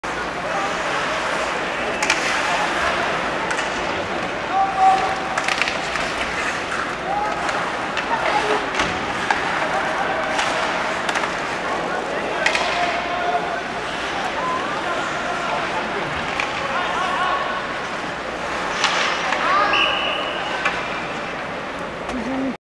Brian Ferlin Nightmare Shift
Brian Ferlin is crushed against the boards and then hit in the face with the puck during the same drill.

Boston Bruins 2015 Training Camp
Sept. 19.  TD Garden - Boston, MA